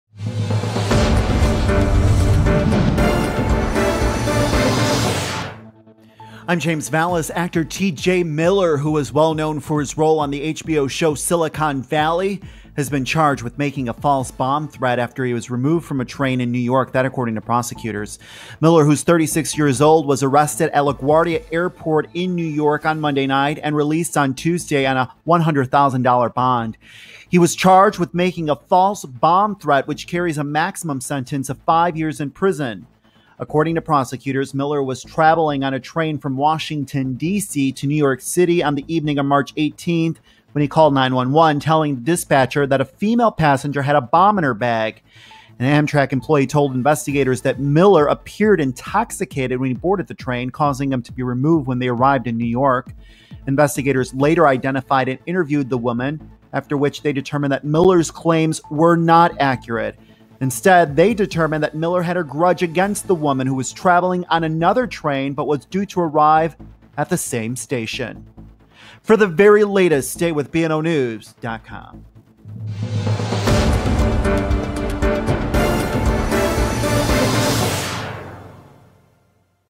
'Silicon Valley' Star T.J. Miller Charged With Making False Bomb Threat
Actor T.J. Miller, who is well known for his role as Erlich on the HBO show “Silicon Valley,” has been charged with making a false bomb threat after he was removed from a train in New York, prosecutors say. James Valles reports. (BNO News)